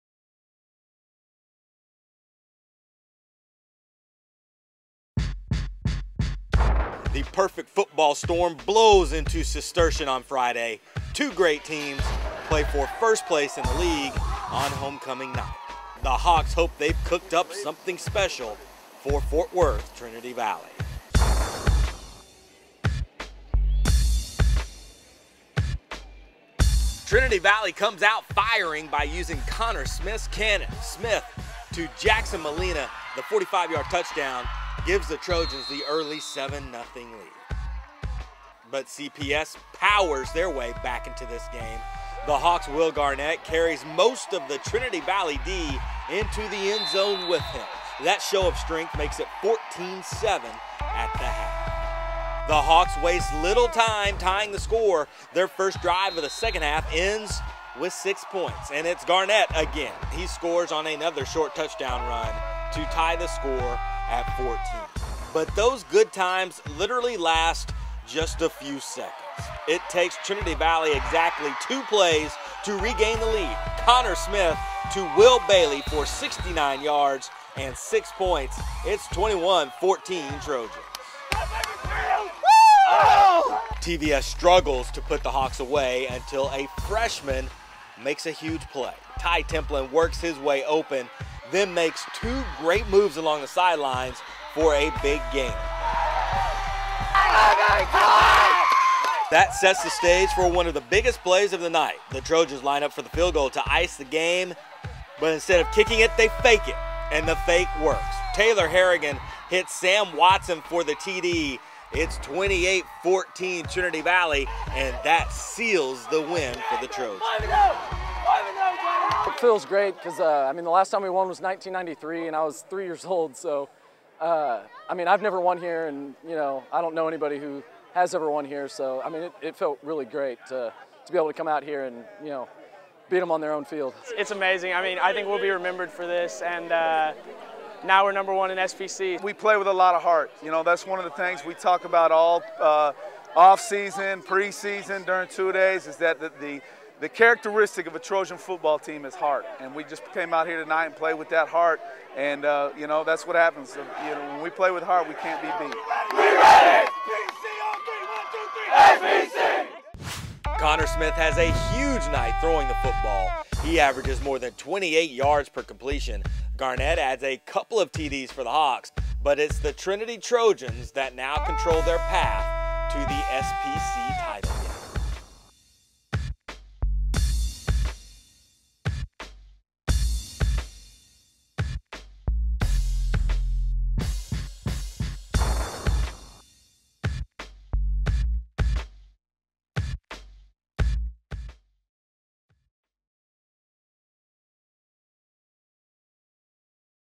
Fort Worth Trinity Valley vs Irving Cistercian
This is the highlight package from LSCSN.COM. Cistercian and Trinity Valley entered the game with undefeated records in the Southwest Prep Conference.